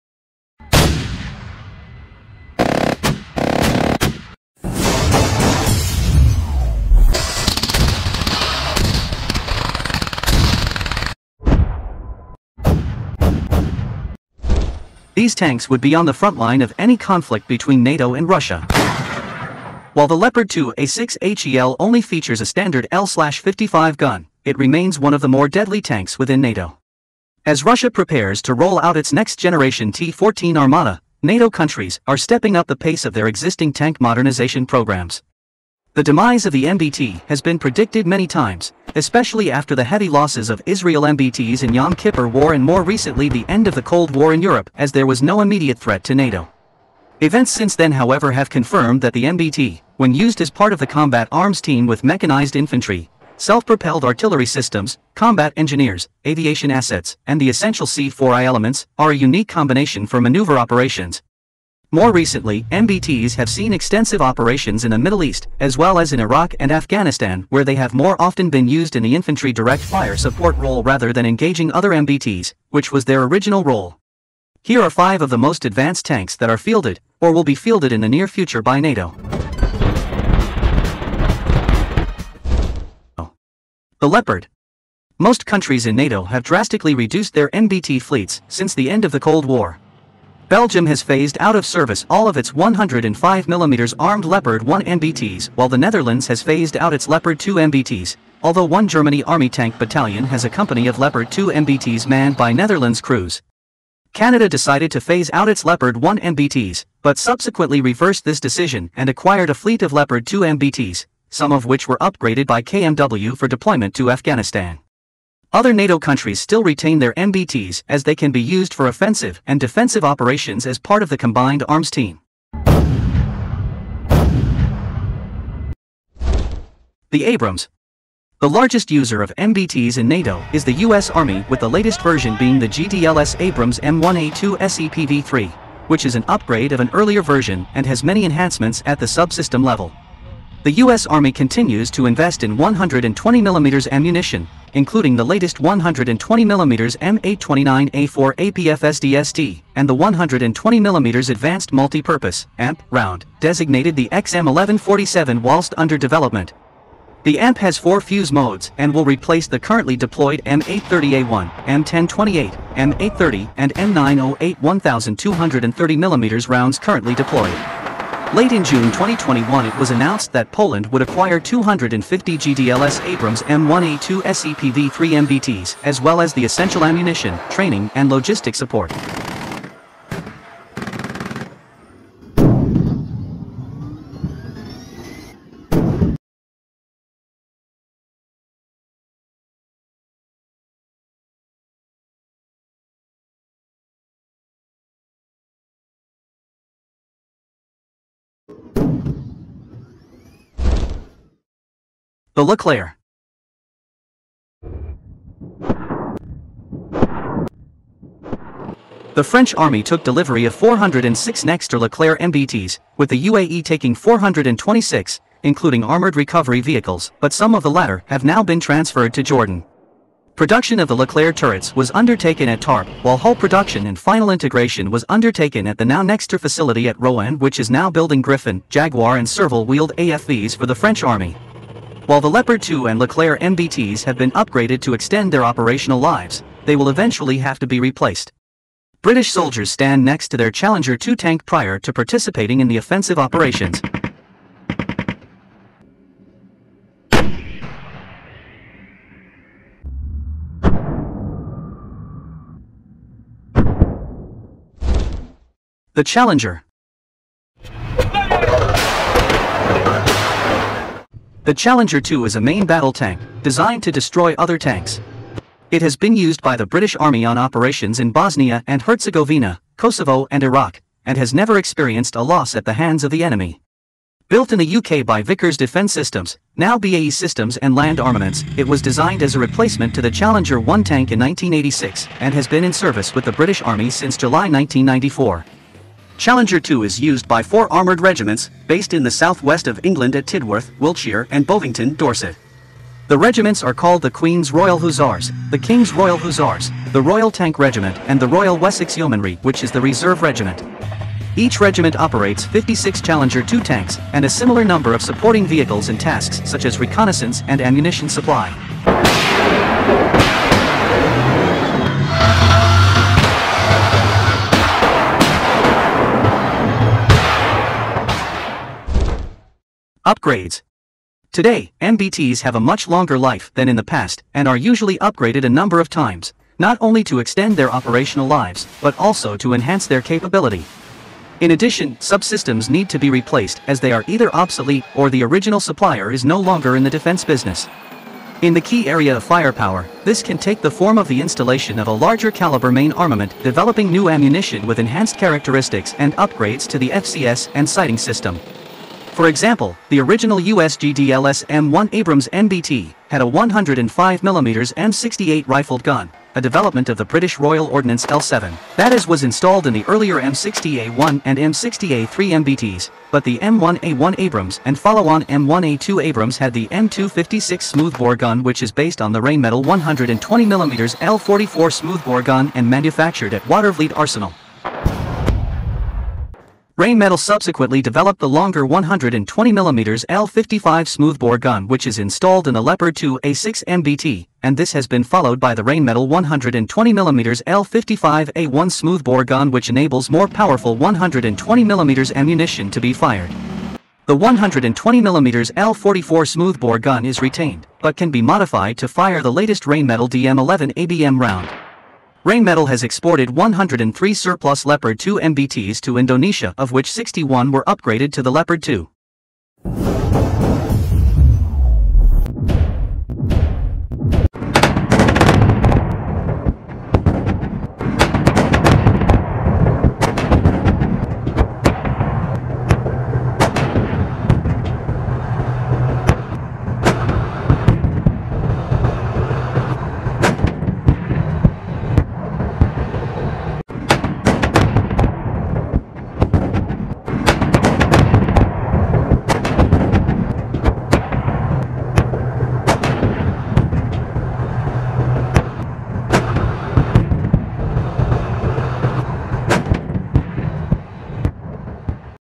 Abrams, Leopard, Leclerc, and Challenger [All of The NATO's Best Tanks in Action]
These tanks would be on the frontline of any conflict between NATO and Russia. While the Leopard 2A6 HEL only features a standard L/55 gun, it remains one of the more deadly tanks within NATO.
As Russia prepares to roll out its next-generation T-14 Armata, NATO countries are stepping up the pace of their existing tank modernization programs.
The demise of the MBT has been predicted many times, especially after the heavy losses of Israel MBTs in Yom Kippur war and more recently the end of the Cold War in Europe as there was no immediate threat to NATO.
Events since then however have confirmed that the MBT, when used as part of the combat arms team with mechanised infantry, self-propelled (SP) artillery systems, combat engineers, aviation assets and the essential C4I elements, are a unique combination for manoeuvre operations. More recently, MBTs have seen extensive operations in the Middle East as well as in Iraq and Afghanistan where they have more often been used in the infantry direct fire support role rather than engaging other MBTs, which was their original role. Here are four of the most advanced tanks that are fielded, or will be fielded in the near future by NATO.
--- Abrams MBT
---Leopard MBT
---Leclerc MBT
--Challenger MBT
Credit: NATO